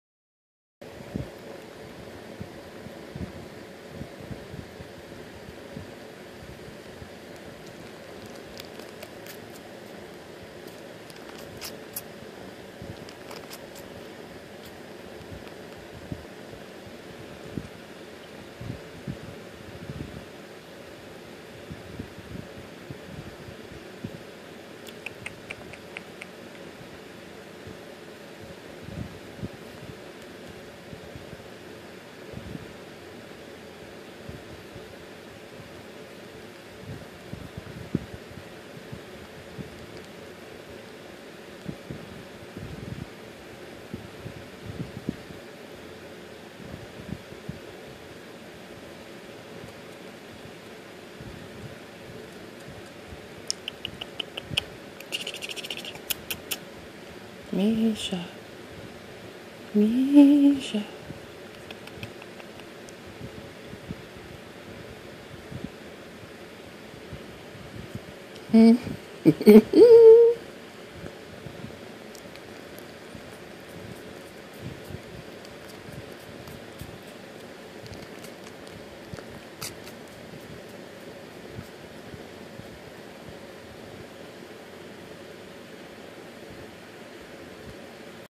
Tabby Munchkin Kitten Sucks Her Paw
Mischa sucks one paw while kneading with the other. She's been doing this from her first day home at 6 weeks old  She's 5 1/2 months old now.